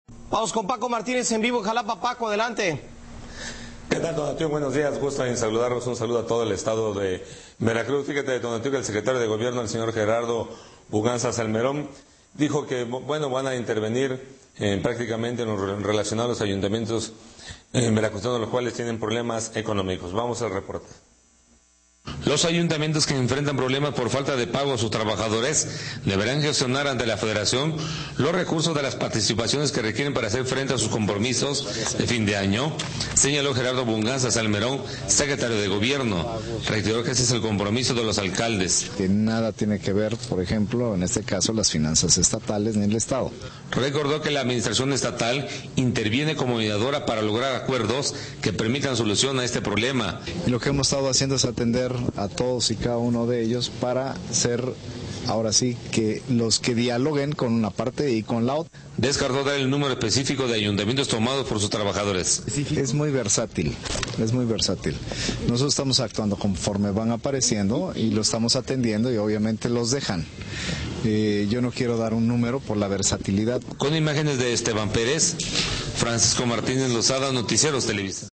Los Municipios en problemas
Por su parte la Secretaría de Gobierno señalo que ante los problemas de liquidez que enfrentan los Ayuntamientos. Estos deberán recurrir a la Federación y no al Estado para resolver sus problemas...